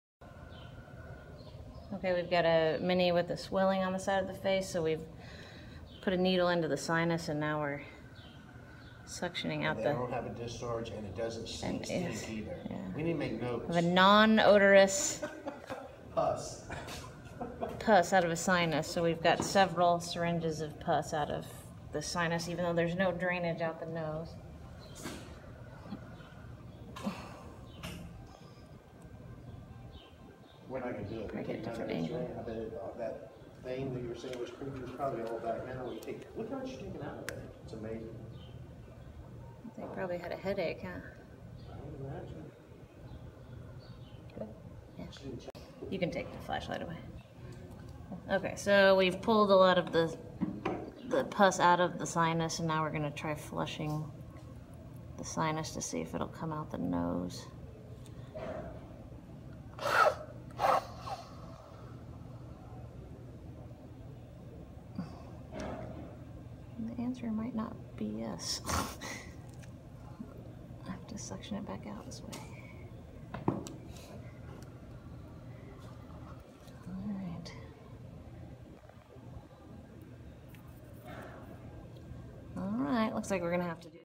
Miniature sinusitis
2 year old miniature horse presented for swelling of the right side of the face under the eye but otherwise acting completely normal.  No nasal discharge.  Radiographs revealed a soft tissue opacity in the maxillary sinus pushing the nasal septum towards the left side.  No dental abnormalities were appreciated on oral exam or radiographs although the horse had some mandibular dental eruption cysts.  The area was numbed and a needle was placed into the maxillary sinus where over 100 cc of non odorous pus was obtained.  No bacteria was isolated.